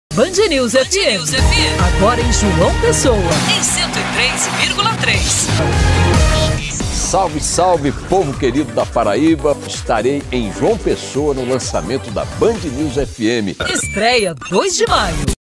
[HD] Chamada Vem Aí Band News FM João Pessoa (versão 2)
Versão pra TV, assim como a rádio tem sua versão.

Captado pelo sinal digital da TV Manaíra. 10.1/16 UHF.